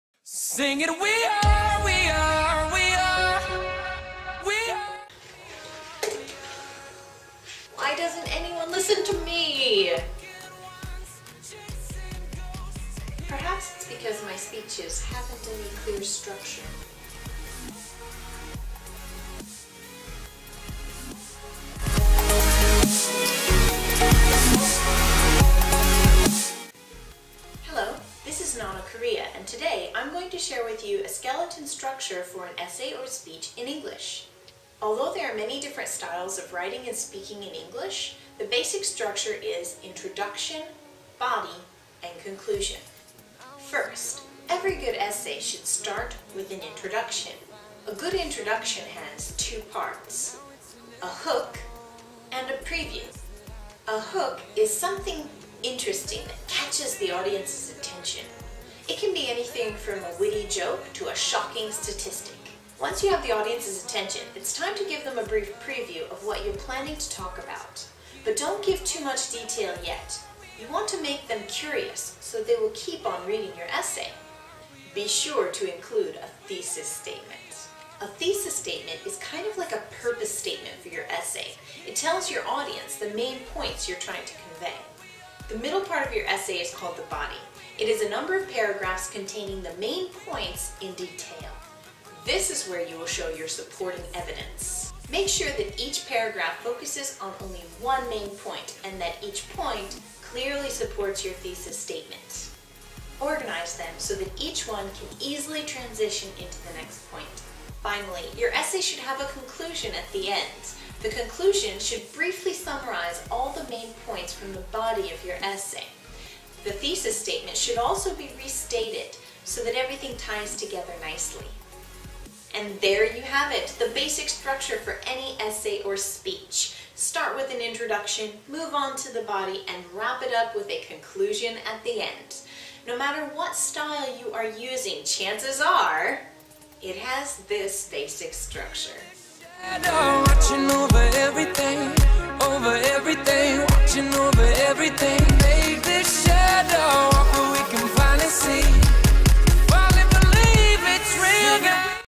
Essay Structure
Basic structure for an essay or speech in English.